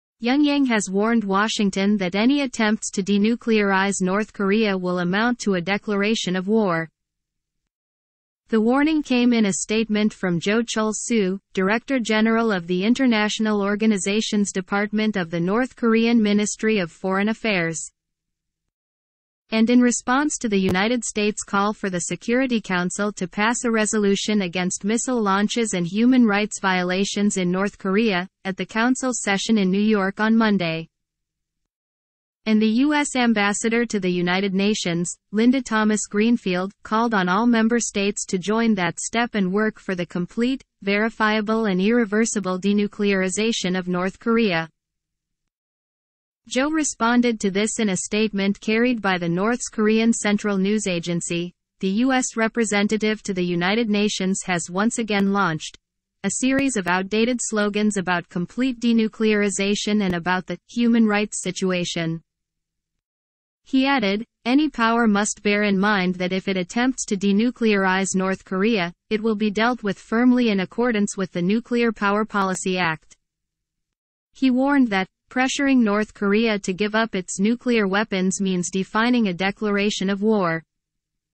North Korea has warned U.S that any attempts to denuclearize it would be a declaration of war.
The warning came in a statement from Jo Chul-soo, Director General of the International Organizations Department of the North Korean Ministry of Foreign Affairs, and in response to the United States' call for the Security Council to pass a resolution against missile launches and human rights violations in North Korea, at the council's session in New York on Monday.